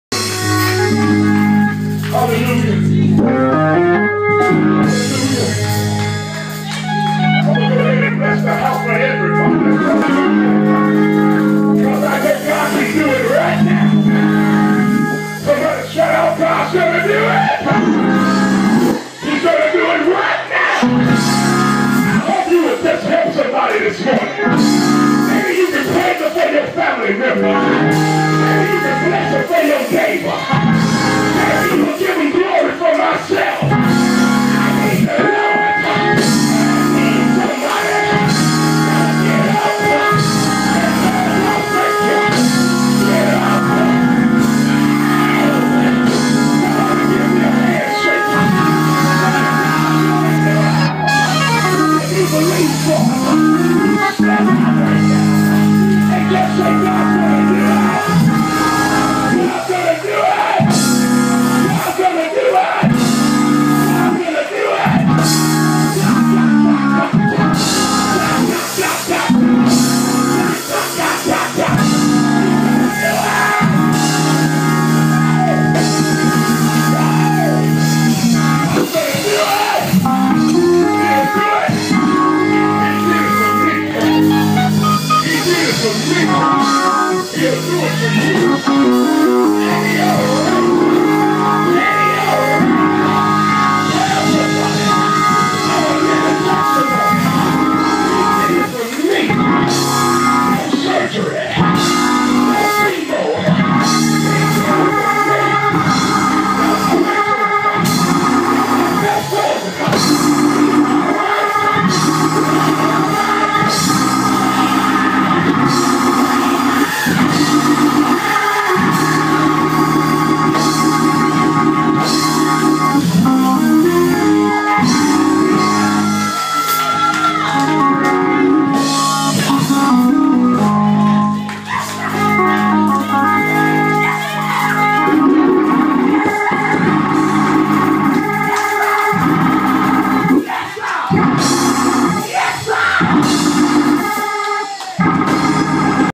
Bigtreal on the organ
Father preaching a couple of Sundays ago